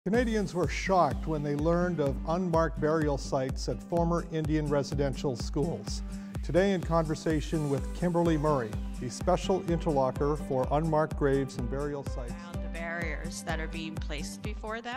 Independent Special Interlocutor for Missing Children and Unmarked Graves
Host Jamie Bramburger is in conversation with Kimberly Murray who is an Independent Special Interlocutor for Missing Children and Unmarked Graves and Burial Sites associated with Indian Residential Schools.
Episode Premieres: June 17th 8pm, encore presentation June 21st 7:30pm